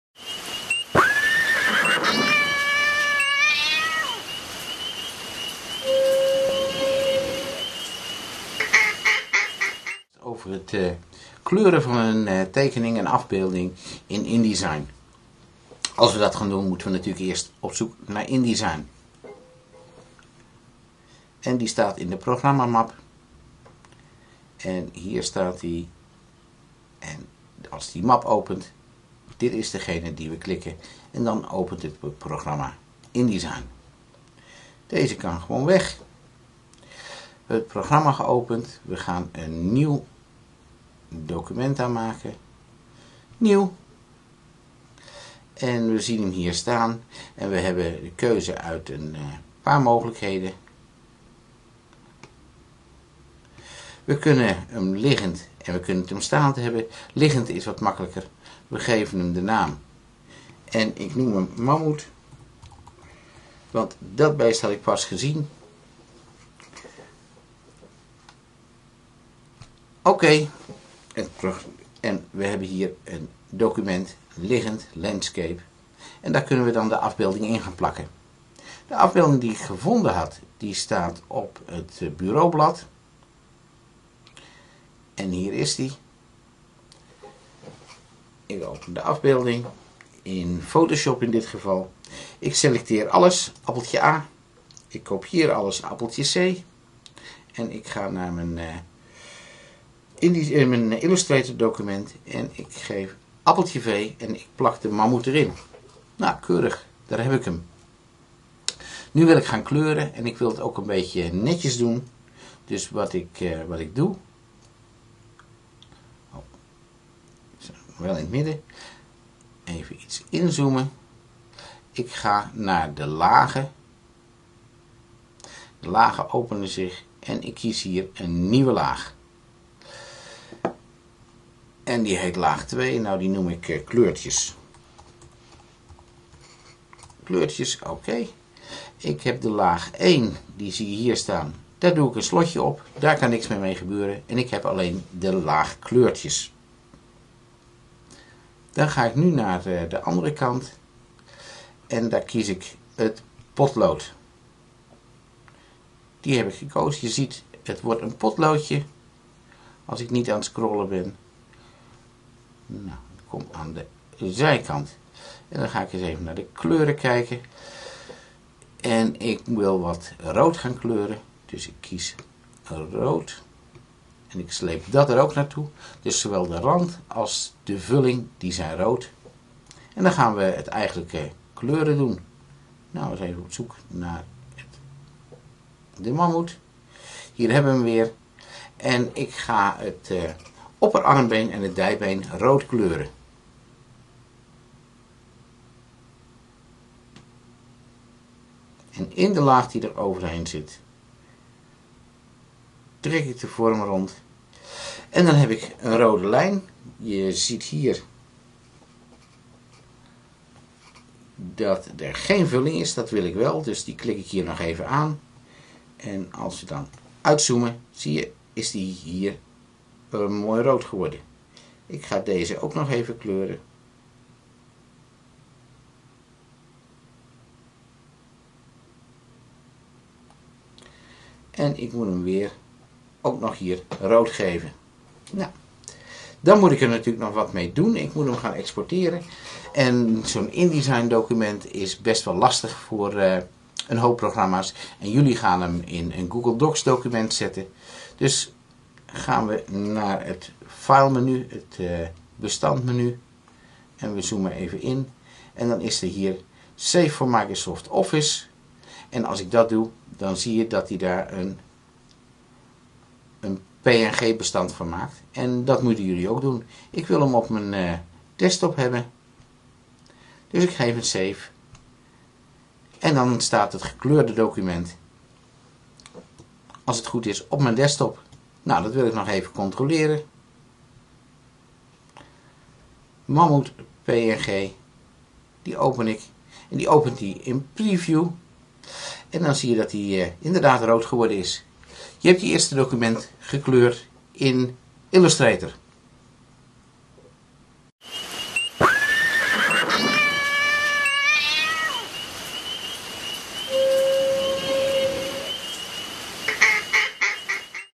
Tekenen in Illustrator podcast
Hoe kleur je de botten van een skelet in Adobe Illustrator